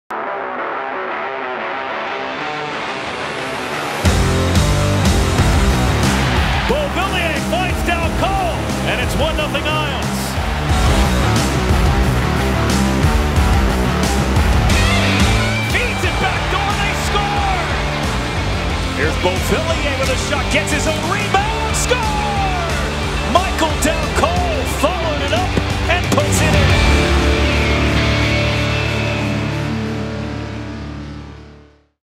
Isles Highlights: Michael Dal Colle
Watch some of Michal Dal Colle's top plays from the 2019-20 season.